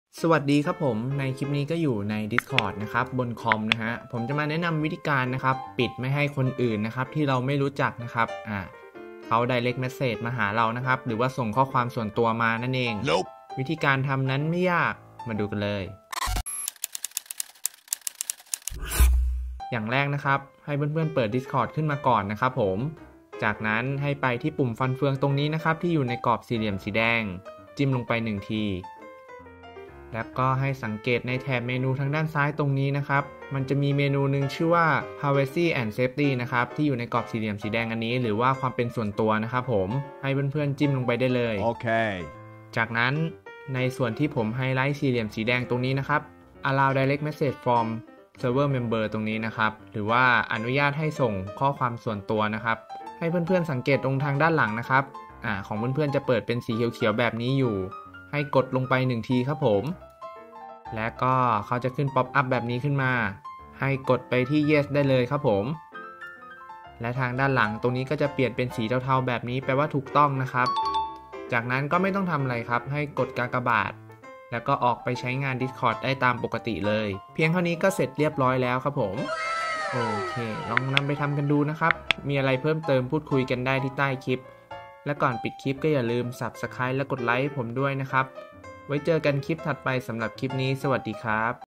วิธีปิดไม่ให้คนอื่นส่งข้อความหาเรา Direct Message PM ในดิสคอร์ด Discord
วิธีปิดไม่ให้คนอื่นส่งข้อความ ข้อความส่วนตัว Direct Message PM หาเราใน ดิสคอร์ด Discord บนคอมง่ายๆใน 1 นาที